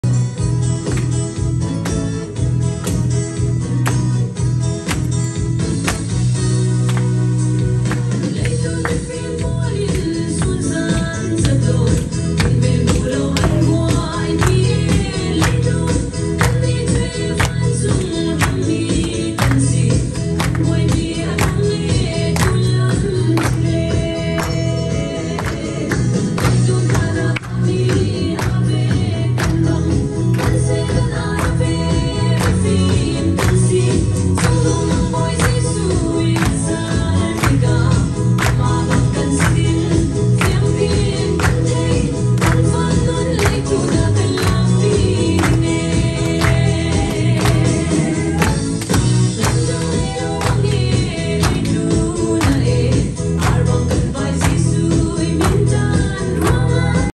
Calvary Mission Church 20th August’23
Worship song in Chin Myanmar language!